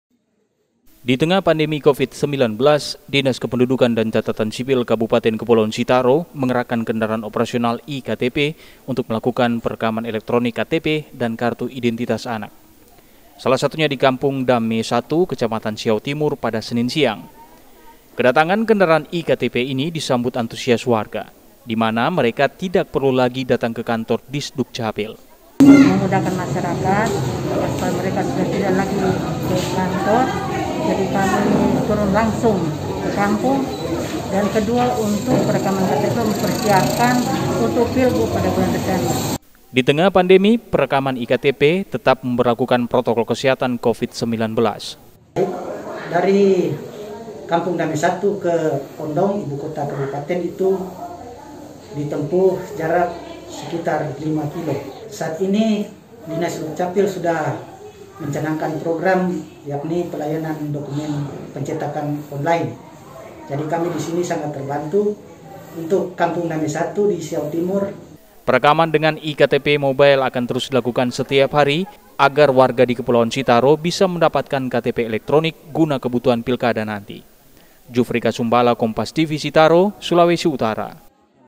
E-KTP Mobile Hingga Ke Pelosok Desa Di Pulau Siau
SITARO, KOMPAS.TV - Dinas Kependudukan Dan Catatan Sipil Kabupaten Kepulauan Sitaro, melakukan  perekaman E-Ktp dan Kia hingga ke pelosok desa di Pulau Siau, Kepulauan Sitaro. Hal ini untuk membantu warga saat pandemic Covid-19, serta meningkatkan jumlah warga memiliki e-ktp jelang pilkada desember mendatang.

Ditengah pandemic Covid-19, Dinas Kependudukan Dan Catatan Sipil Kabupaten Kepulauan Sitaro, mengerahkan kendaraan operasional E-Ktp  untuk melakukan perekaman elektronik ktp dan kartu identitas anak.

Salah satunya di Kampung Dame Satu, Kecamatan Siau Timur, pada Senin siang, 7 September 2020. Kedatangan kendaraan E-Ktp ini disambut antusias warga, dimana mereka tidak perlu lagi datang ke kantor disdukcapil.

"saya mewakili warga desa sangat bersyukur karena sudah ada kendaraan perekaman e-ktp yang melayani hingga ke pelosok desa. ini sangat membantu warga karena tidak perlu lagi datang ke kantor disdukcapil, yang jaraknya cukup jauh," ujar Herry Bukasiang, Kapitalau Dame Satu.

Ditengah pandemi, perekaman E-Ktp dengan ini, tetap memberlakukan protokol kesehatan covid-19. perekaman dengan e-ktp mobile akan terus dilakukan setiap hari agar warga dikepulauan sitaro bisa mendapat ktp elektronik guna kebutuhan pilkada nanti.

JUFRI KASUMBALA / KOMPAS TV / SITARO / SULAWESI UTARA //

Sulawesi Utara